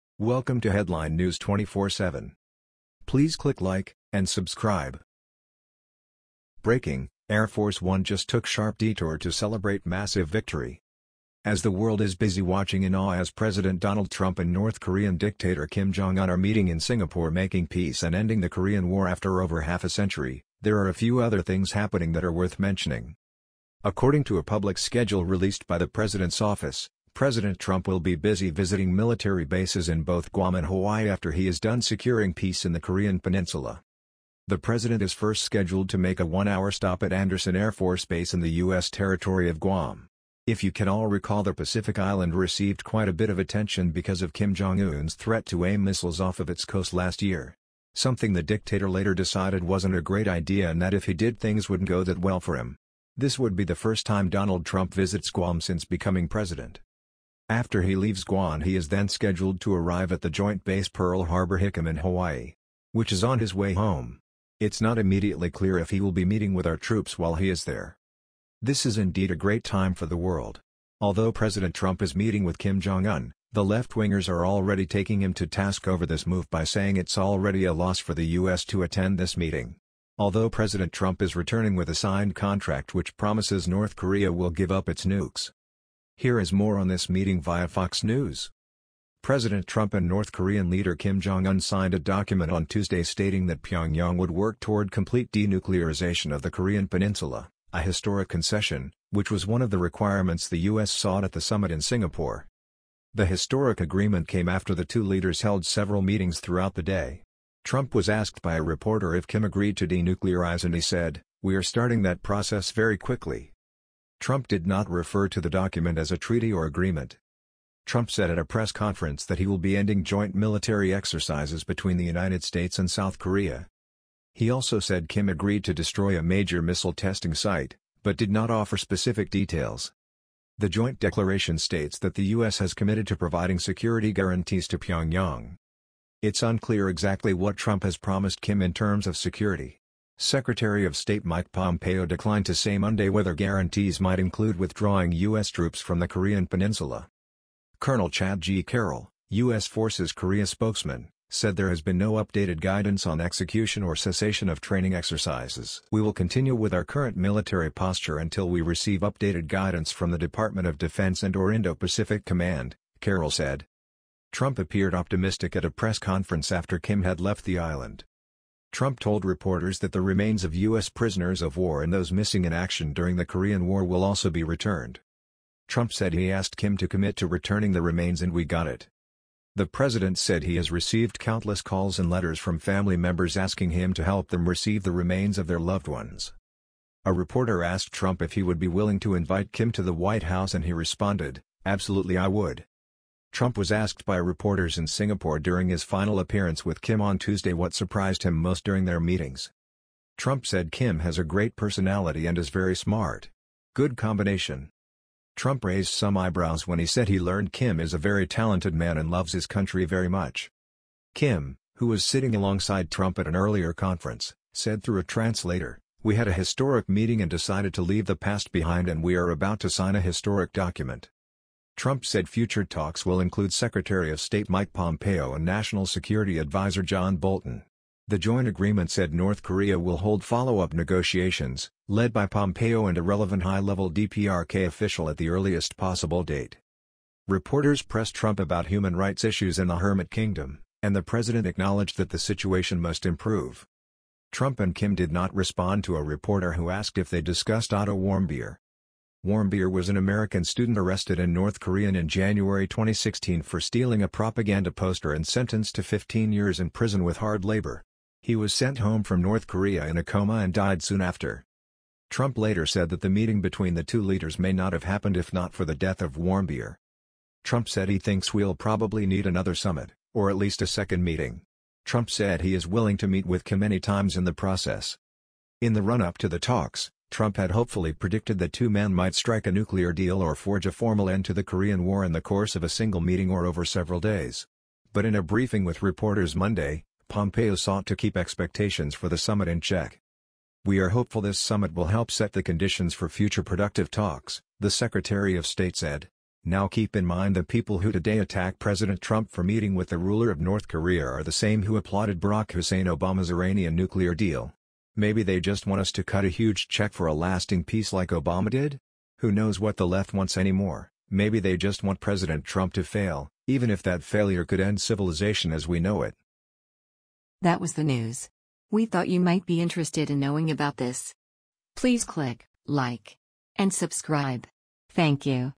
BREAKING: Air Force One Just Took Sharp Detour To Celebrate Massive Victory!
President Trump and North Korean leader Kim Jong Un signed a document on Tuesday stating that Pyongyang would work toward “complete denuclearization of the Korean Peninsula”— a historic concession, which was one of the requirements the U.S. sought at the summit in Singapore.